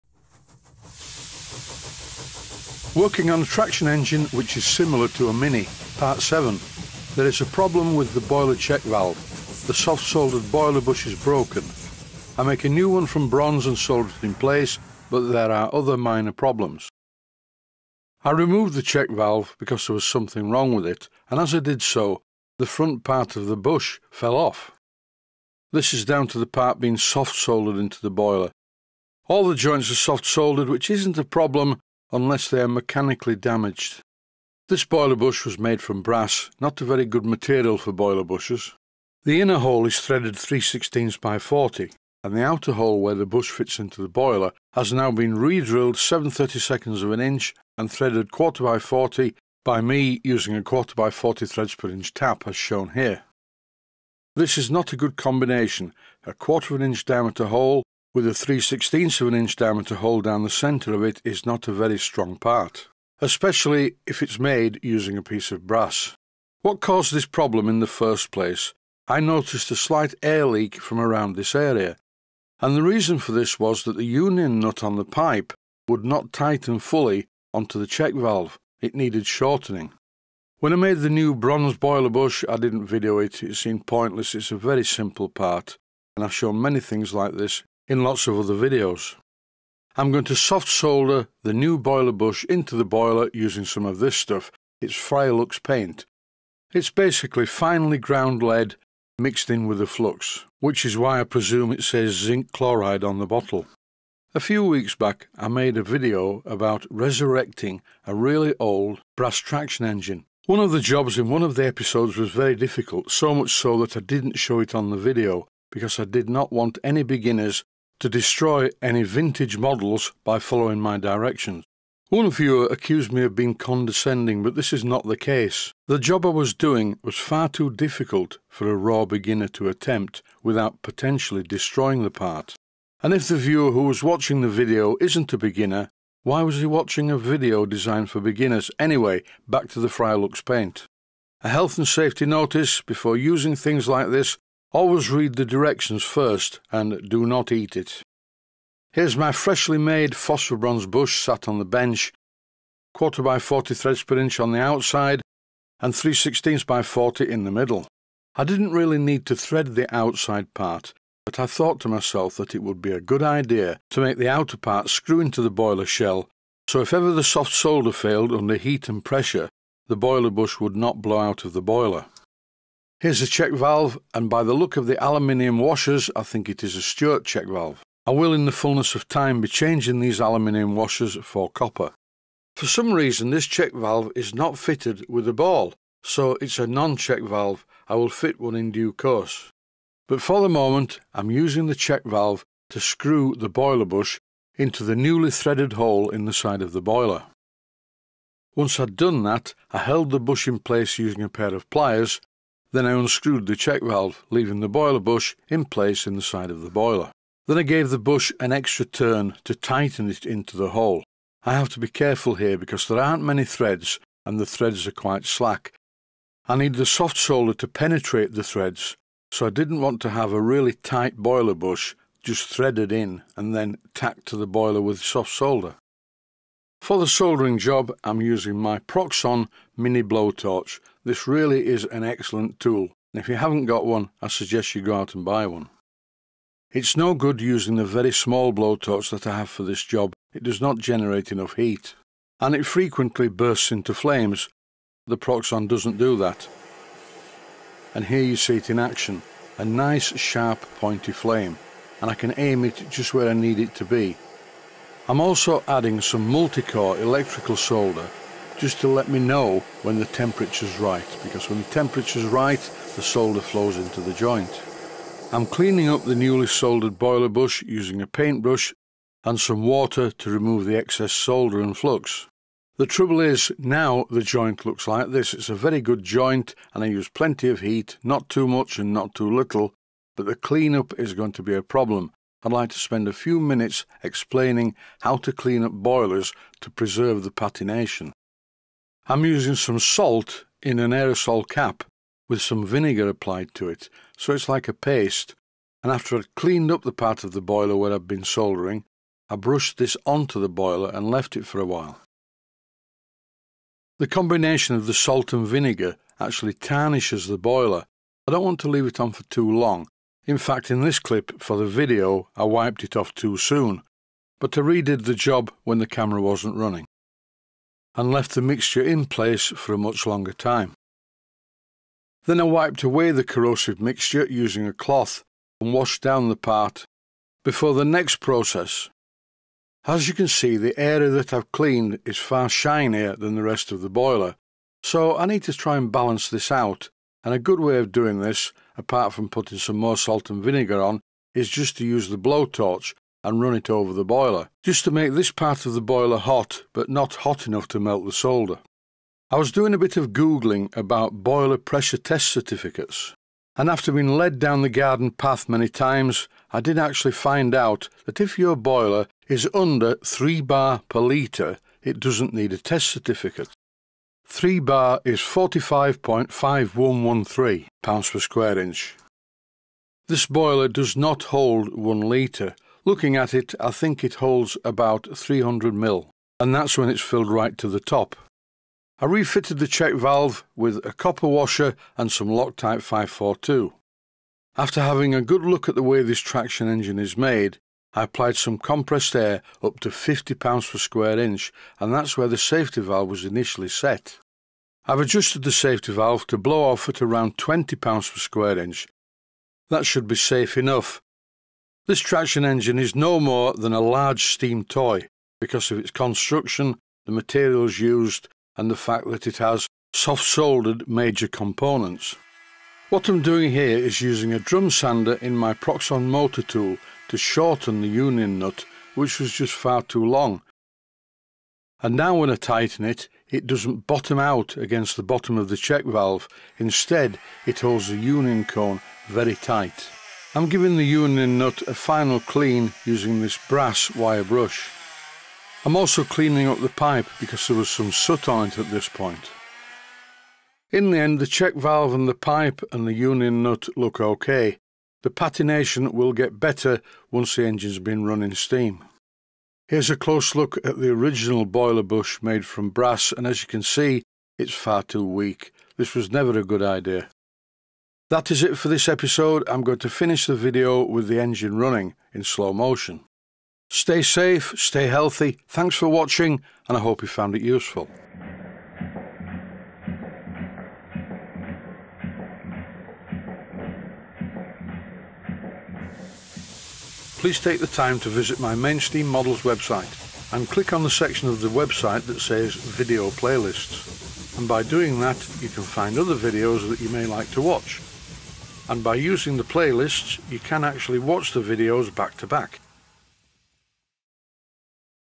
A "MINNIE" TYPE TRACTION ENGINE - PART #7
A "Minnie" Type Traction Engine - Part #7. There is a problem with the Boiler Check Valve. The Soft Soldered boiler bush is broken. I make a new one from Bronze and solder it in place but there are other minor problems.